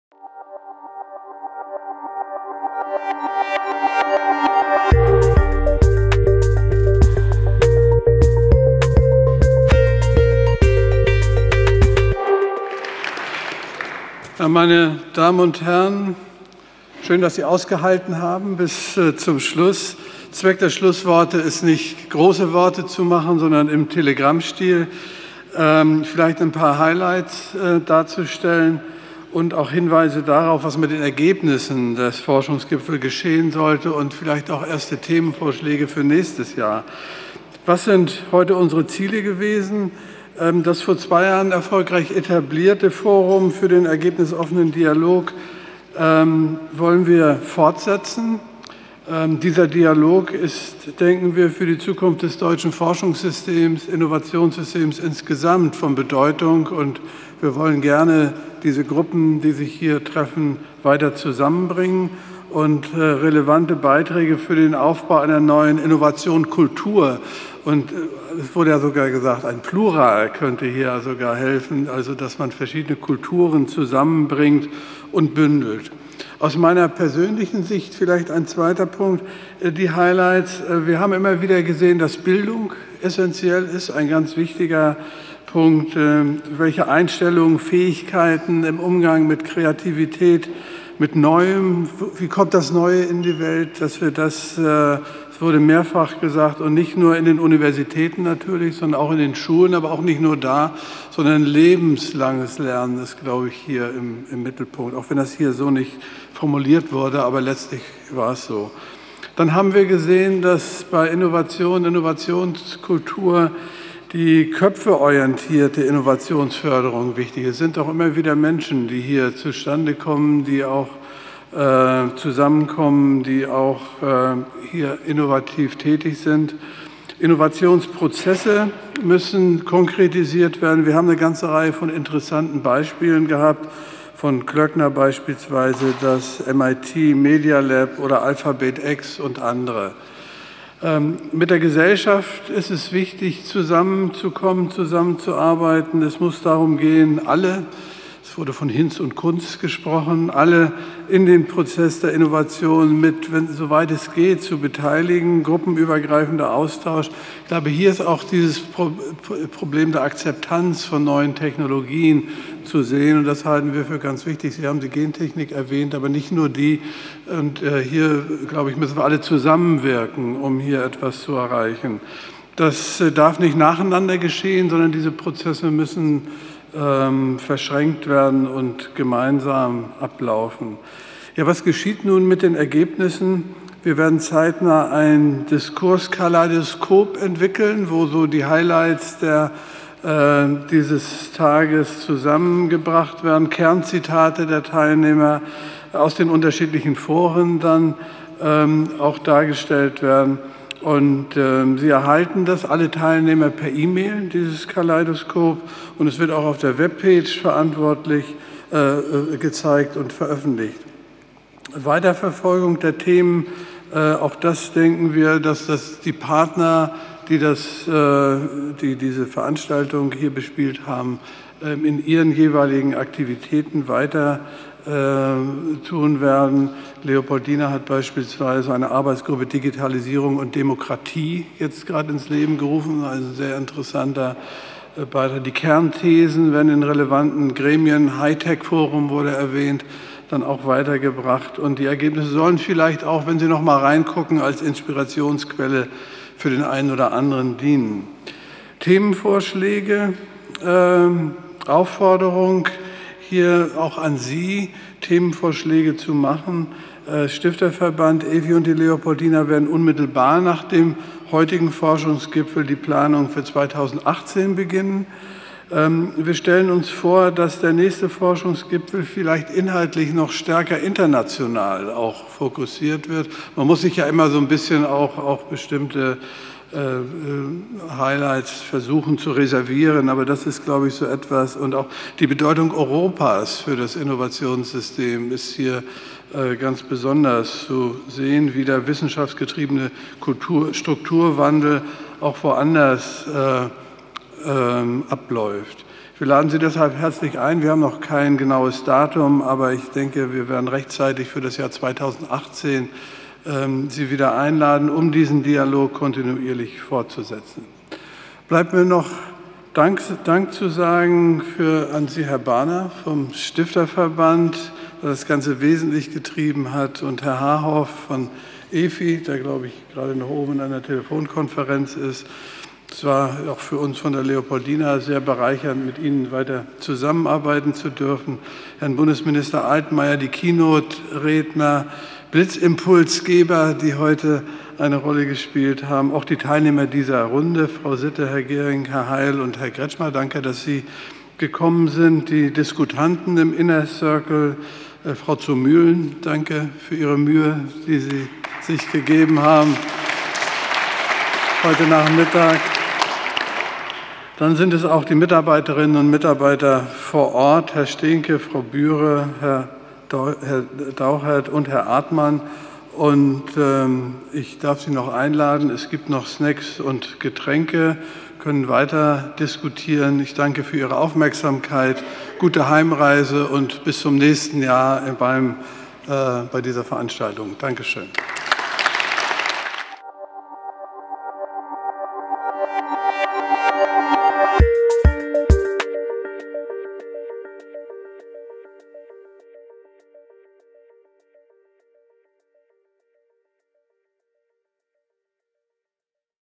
Forschungsgipfel 2017 - Abschlussworte Jörg Hacker
Abschlussworte
Prof. Dr. Jörg Hacker, Präsident der Nationalen Akademie der Wissenschaften Leopoldina, Halle an der Saale

am 28. März 2017 in Berlin